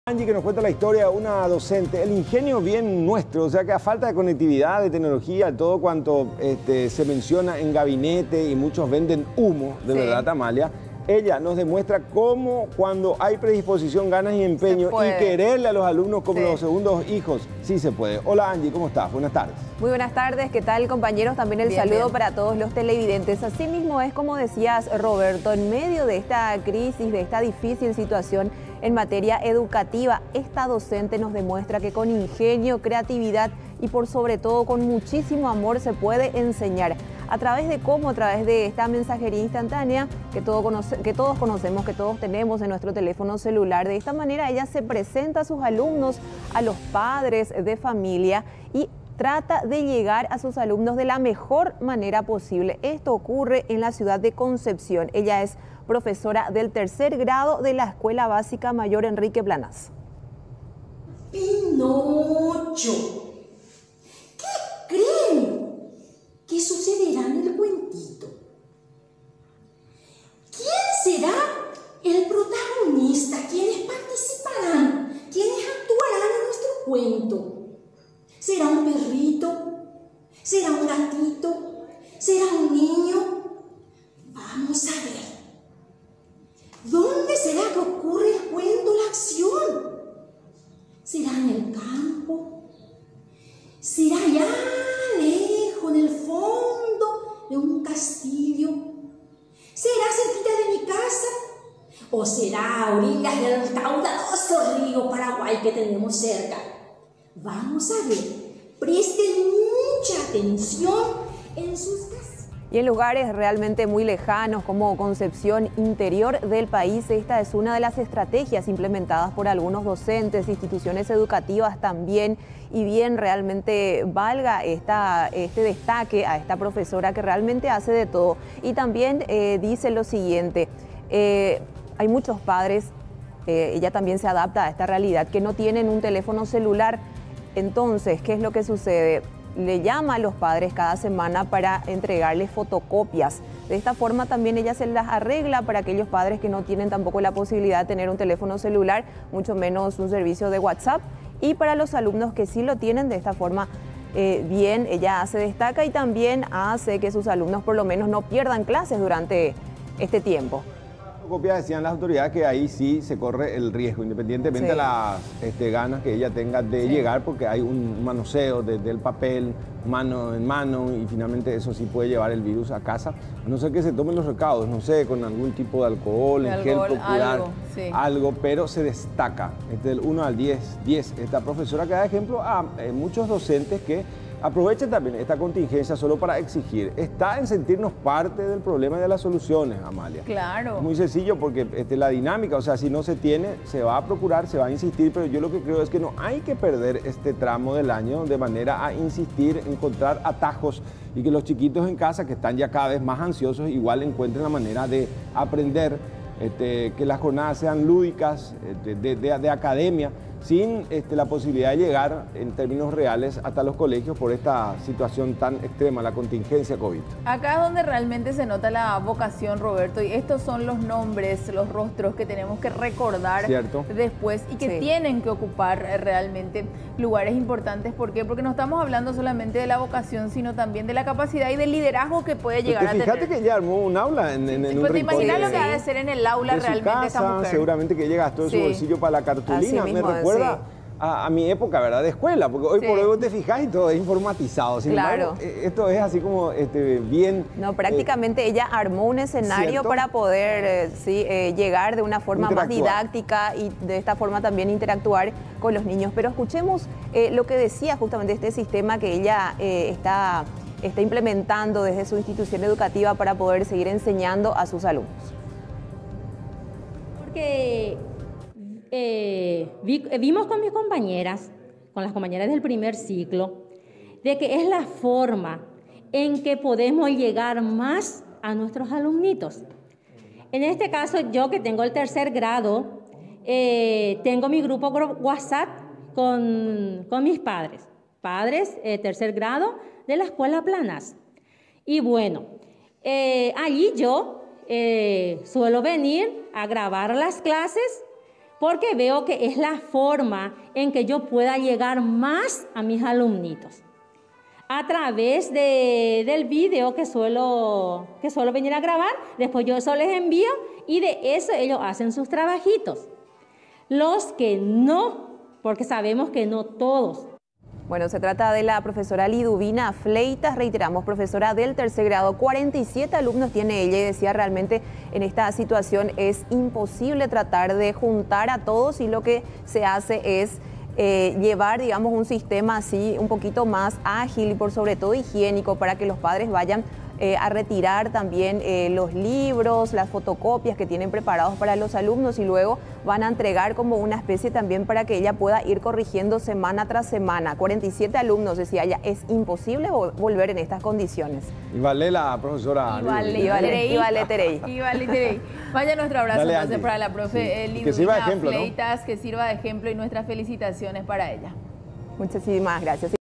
Maestra enseña por WhatsApp en Concepción
Llega a sus alumnos mediante la aplicación

 Con ingenio, creatividad y amor hacia la docencia, la Profe Liduvina Fleitas realiza vídeos para sus alumnitos del 3er. grado y se los envía a los padres para que no pierdan las clases. ▂▂▂▂▂▂▂▂▂▂▂▂▂▂▂▂▂▂▂▂▂